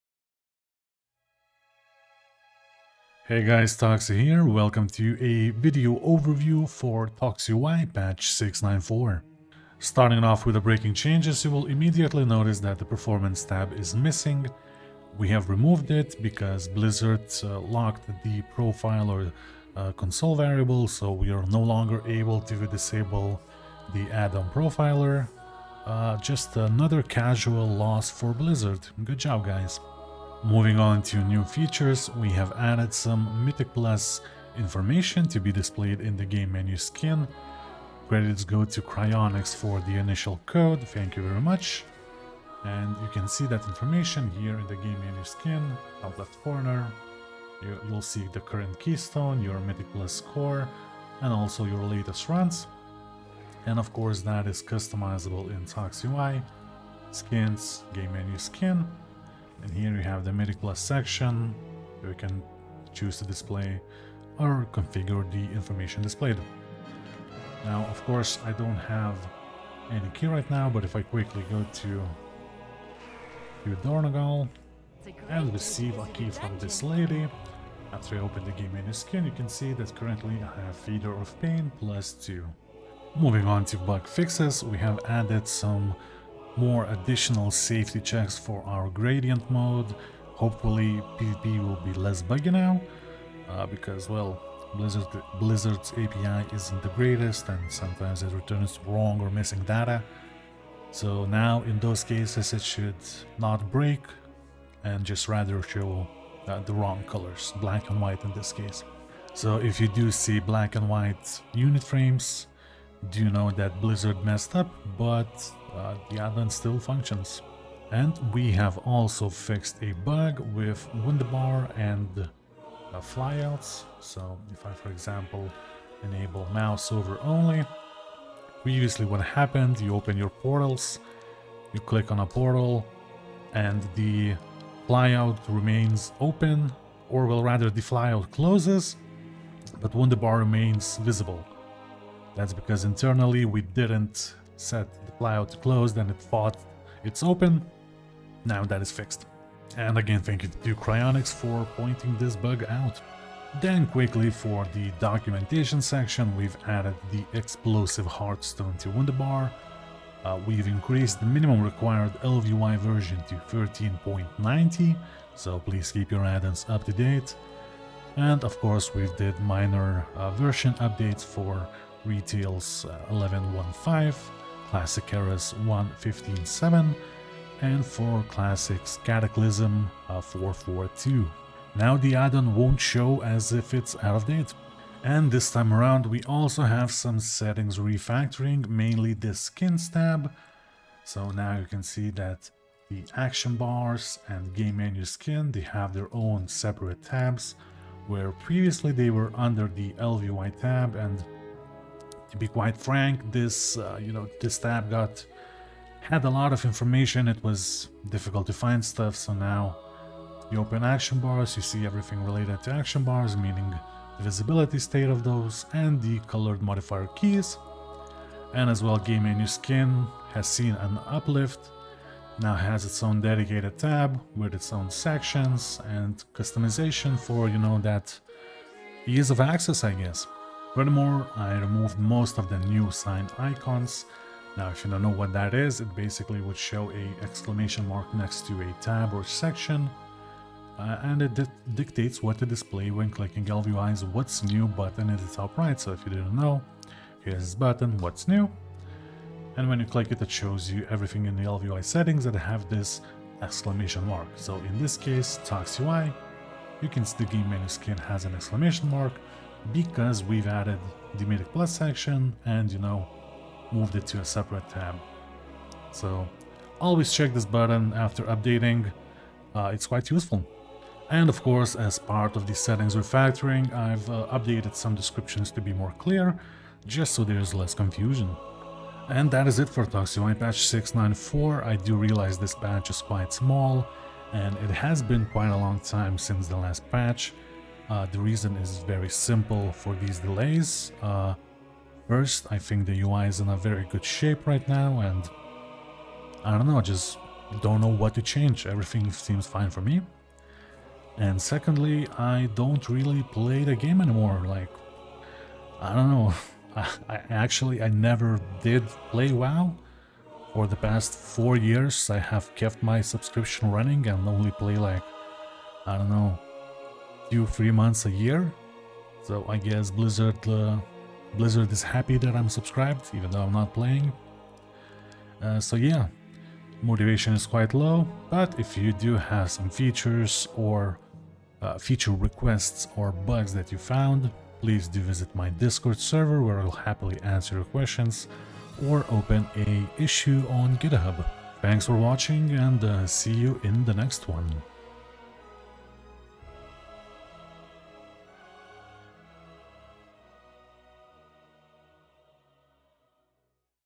ToxiUI Patch 6.9.4
Going over the changelog for ToxiUI's Patch v6.9.4

Timestamps
00:00 Intro
00:10 Breaking changes
00:30 New features
01:23 Bug fixes
02:33 Documentation
03:05 Settings refactoring
04:39 Outro